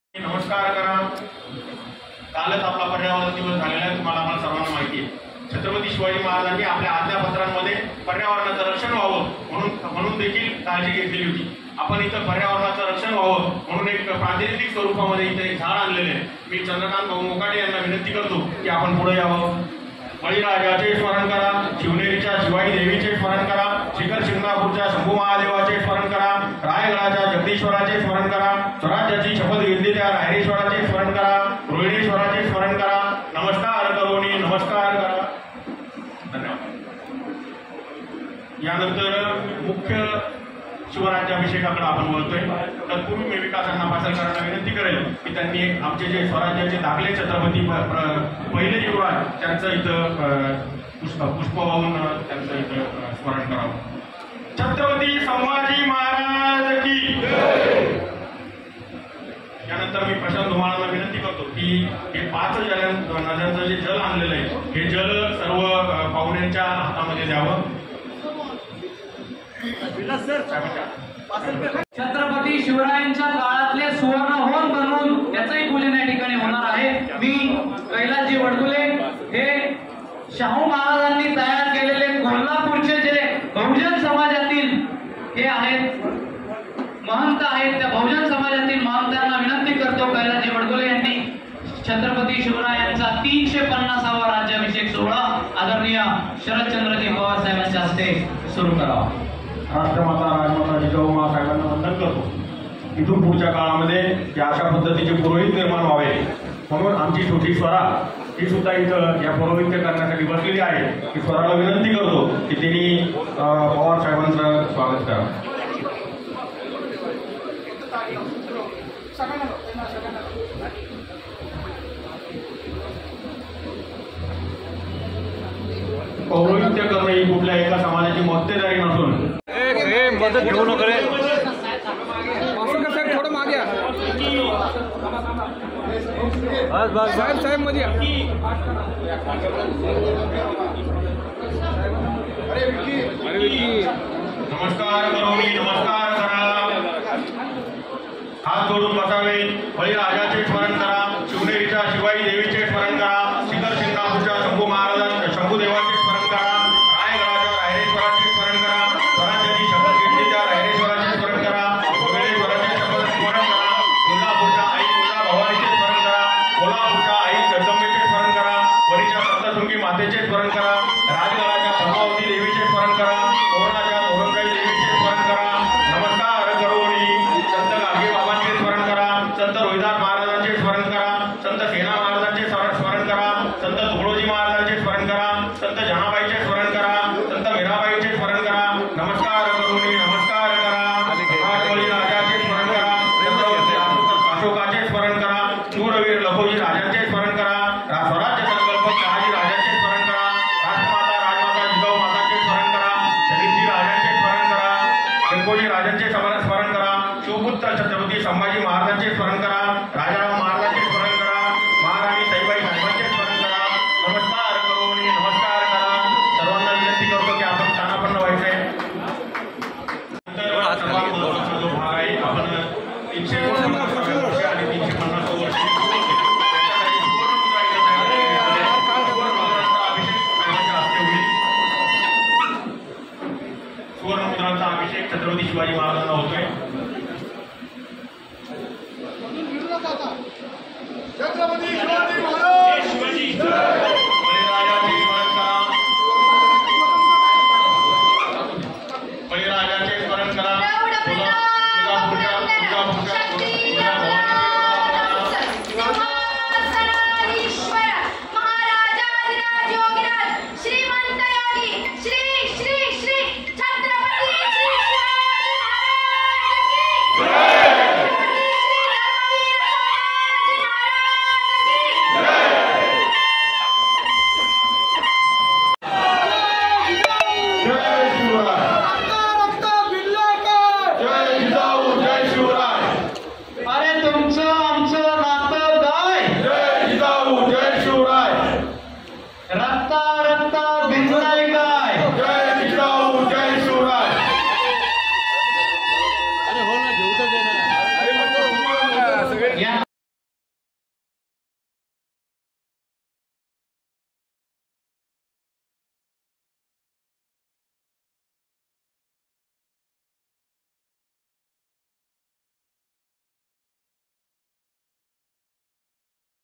Video | लाल महालात शिवरायांचा राज्याभिषेक, पाहा! | LetsUpp Marathi
पुण्यातील ऐतिहासीक लाल महालात छत्रपती शिवरायांचा राज्याभिषेक करण्यात आला. या कार्यक्रमासाठी राष्ट्रवादीचे सर्वेसर्वा शरद पवार यांची उपस्थिती होती.